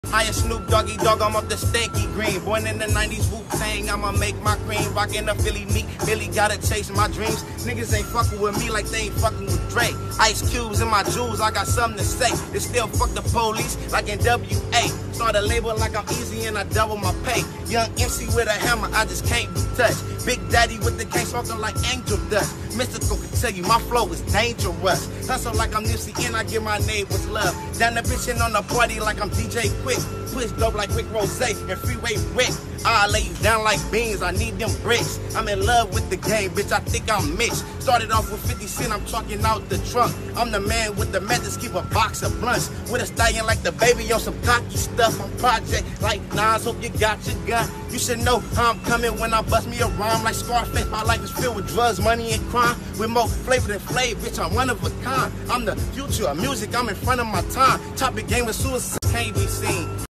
Yung Woo Freestyles Paying Homage To Hip Hop Rap Artists Who Inspired Him
Own Tha Block. News